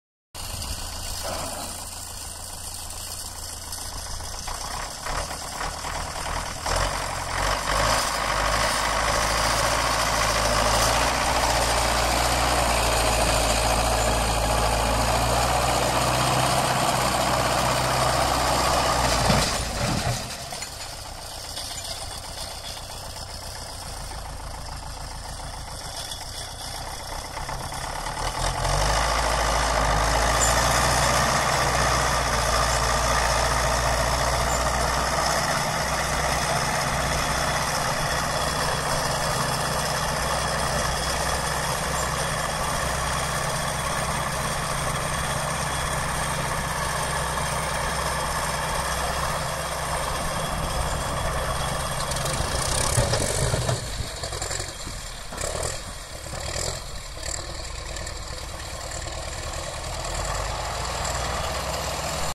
Awesome Mtz vs Dt 75(start up)!
Mtz push starts  Dt 75